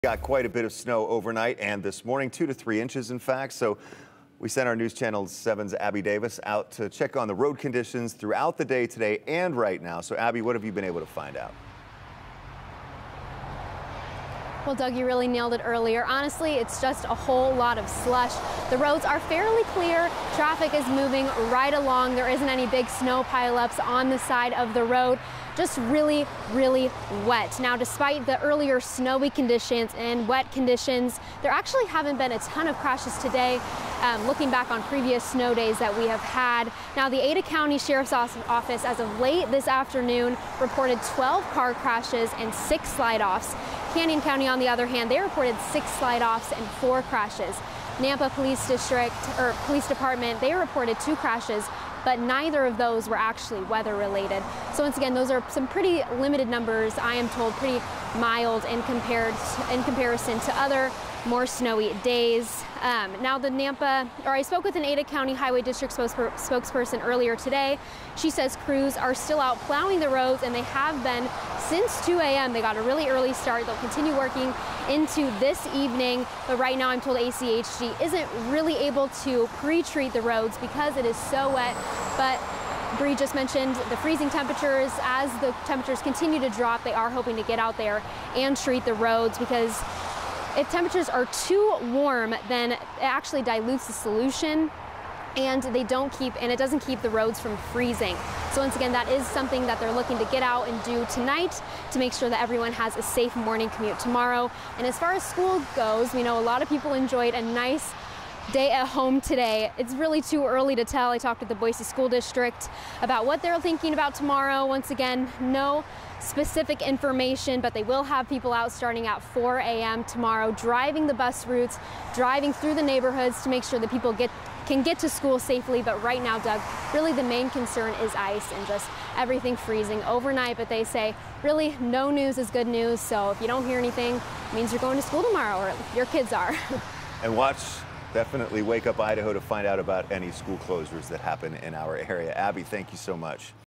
Road conditions in the valley for Monday, Dec. 12
There haven't been a lot of crashes but there is a lot of slush and wet conditions.